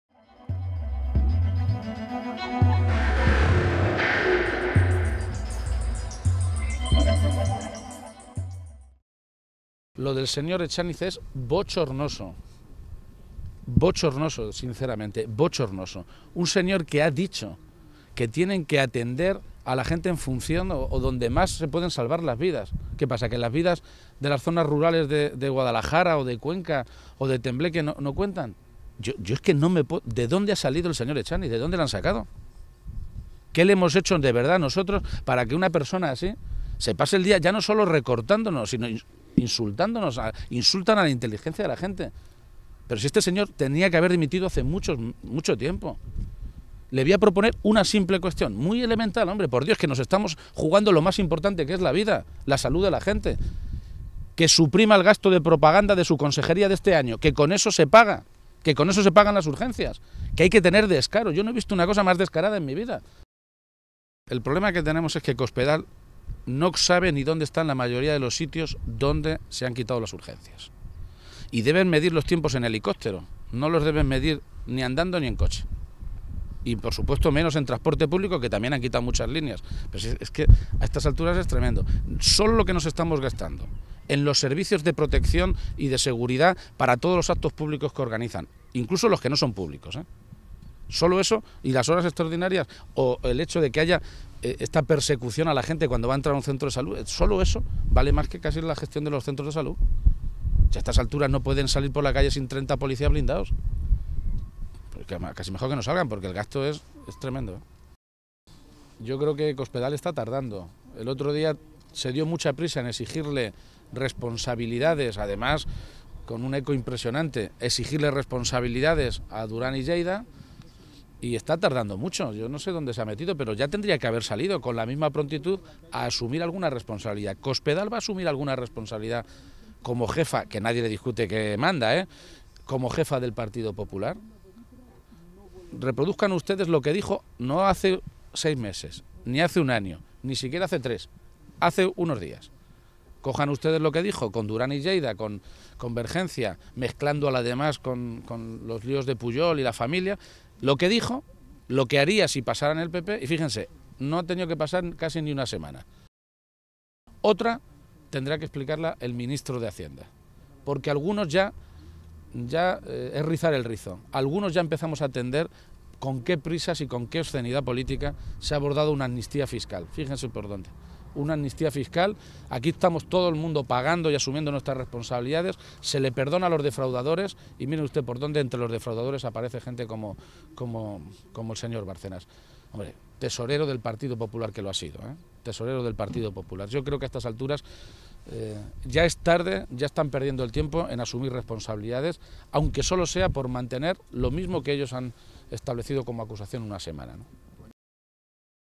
Echániz debería haber dimitido: No solo recorta, sino que insulta y nos toma a todos por tontos
El secretario general del PSOE de Castilla-La Mancha, Emiliano García-Page, ha calificado hoy como "bochornosas", las declaraciones efectuadas ayer por el consejero de Sanidad del Gobierno regional, José Ignacio Echániz, que para justificar el cierre de las urgencias nocturnas en 21 centros de salud de la región señaló que "los médicos hay que ponerlos donde salvan más vidas".

García-Page se ha preguntado "de dónde han sacado a alguien como Echániz, que dice estas cosas y que hace ya mucho tiempo que debería haber dimitido, porque además de recortar la Sanidad de la manera en la que lo está haciendo, nos insulta y nos toma a todos los castellano-manchegos por tontos".

A continuación, cuestionaba si para el consejero "las vidas en los pueblos de Cuenca, de Guadalajara o en Tembleque valen menos". Y recordaba el largo "historial" de declaraciones, cuanto menos, desafortunadas del propio Echániz: "Cuando protestaron los farmacéuticos les llamó de todo, a los médicos de las urgencias, para justificar el cierre, les dijo que estaban todo el día durmiendo, a los pensionistas, que el copago en las medicamentos eran cuatro cafés...De verdad, ¿de dónde han sacado a Echániz?", criticaba.

Además, el líder de los socialistas castellano-manchegos insistía en que "nadie debe dejarse engañar. Le vuelvo a proponer al señor Echániz la solución: Con lo que va a aumentar este año el dinero que se gastan en propaganda en su consejería, se mantienen las urgencias abiertas".

Page se refería también al hecho de que algunos alcaldes del PSOE en los pueblos donde se cierran las urgencias hayan recurrido a notarios para certificar que el tiempo de traslado a los nuevos puntos de atención superan la media hora que fija la Ley. A este respecto, aseguraba que "el verdadero problema que tenemos es que Cospedal no sabe dónde están los pueblos en los que ha cerrado las urgencias, y han medido el tiempo en helicóptero, no en coche. Solo con lo que nos cuesta el servicio de seguridad de los actos públicos, mantenemos las urgencias".

Bárcenas y la medicina que Cospedal recetó a Durán i Lleida

A preguntas de los periodistas, García-Page se ha referido también a las informaciones, conocidas hoy, sobre que Luis Bárcenas, siendo tesorero nacional del PP, hubiera presuntamente evadido 22 millones de euros a Suiza y luego regularizado 10 millones en la amnistía fiscal aprobada por el Gobierno del PP el año pasado.

En este sentido, ha preguntado a Cospedal "cuánto tiempo va a tardar en asumir responsabilidades como secretaria general del PP nacional", y ha pedido coherencia para que "Cospedal se aplique a ella misma la medicina que le recetó hace apenas diez días a Durán i Lleida".